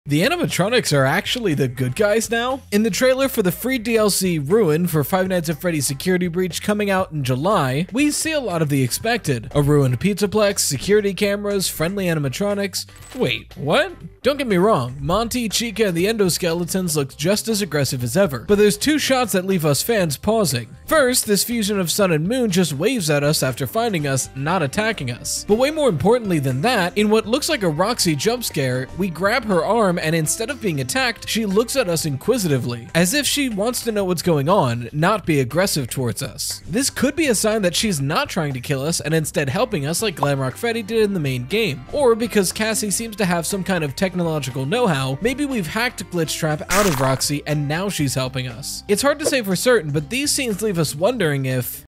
FNAF Animatronics are good guys now?! | fnaf theroy
Get your own apron!

The style of this video is similar to Fuhnaff, Pastra,  FusionZGamer, and uhyeah. A large influence on this channel has been Wendigoon, Game Theory, Markiplier, and Dawko.